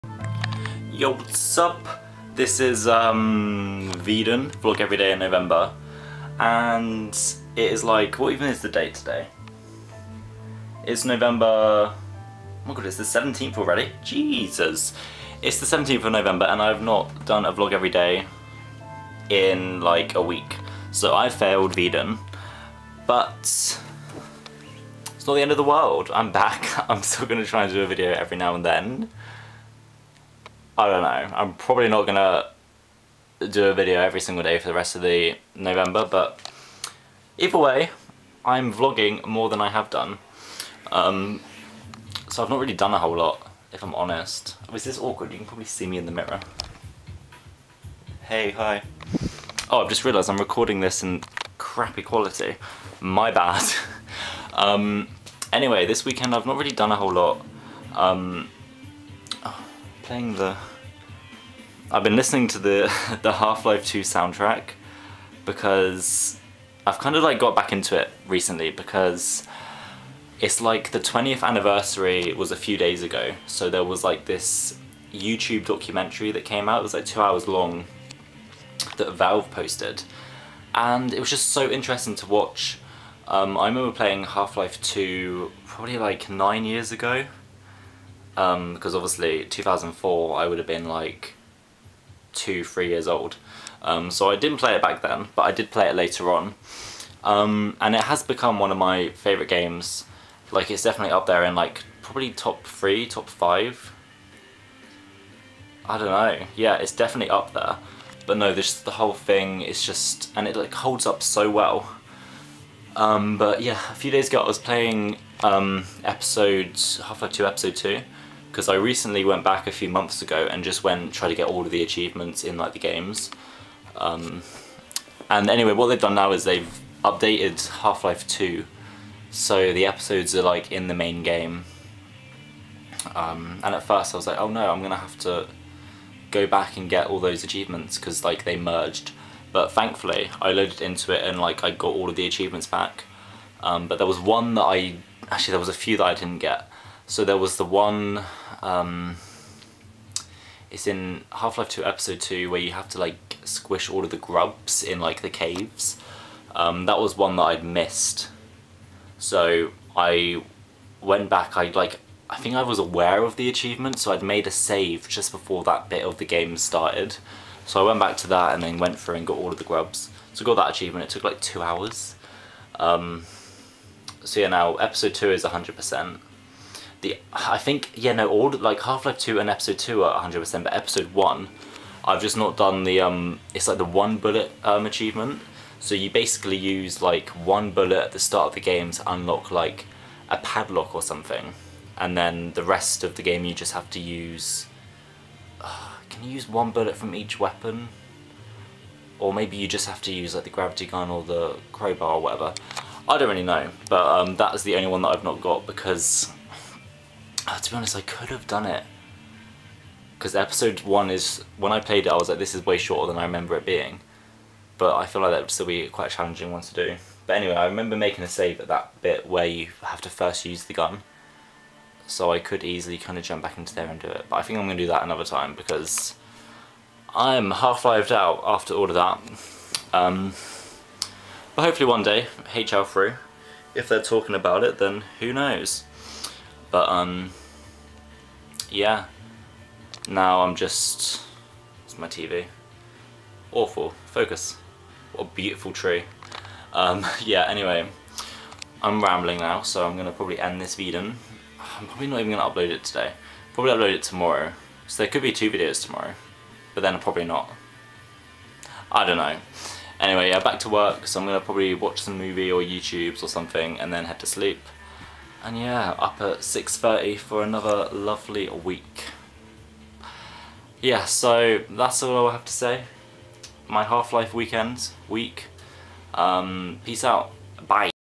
Half-Life 2 anniversary
VEDN day 11: I FAILED VEDN and I love Half-Life :D

QOTV: Have you played any of the Half-Life games?